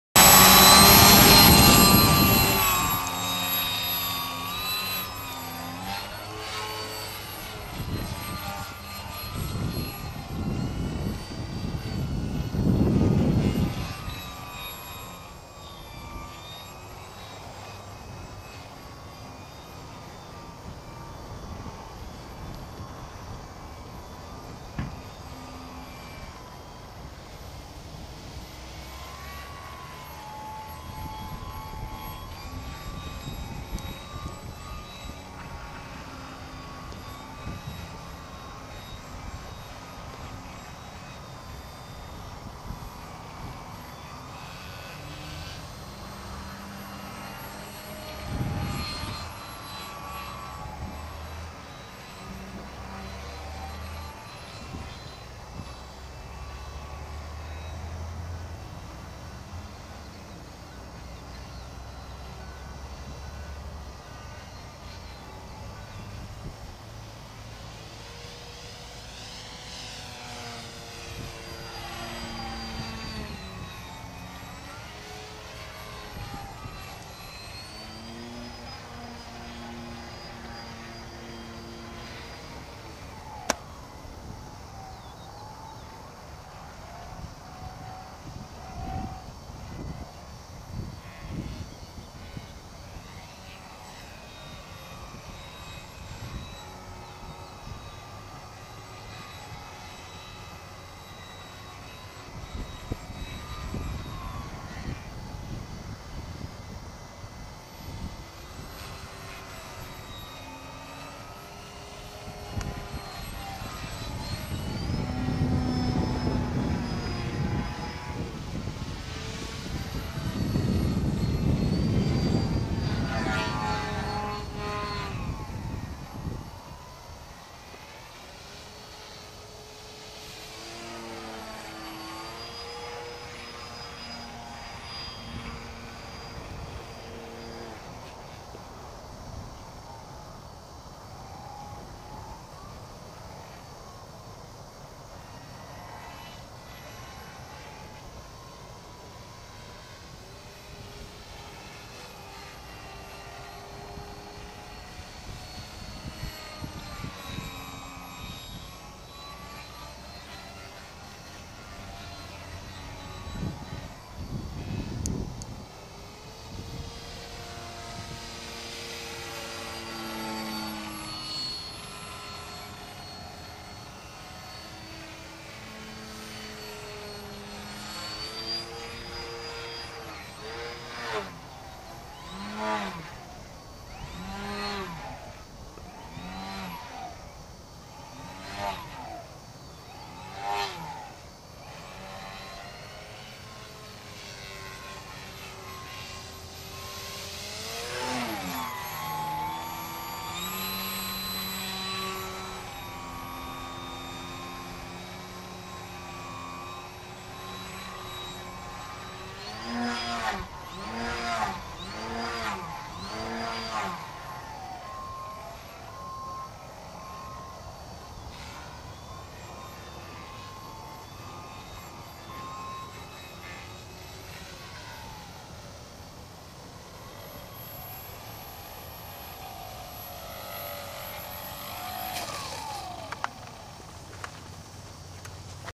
Hod Rod Havin Fun
39 inch wing 18ozs 'Hot Rod ' foamy electric 3D rc airplane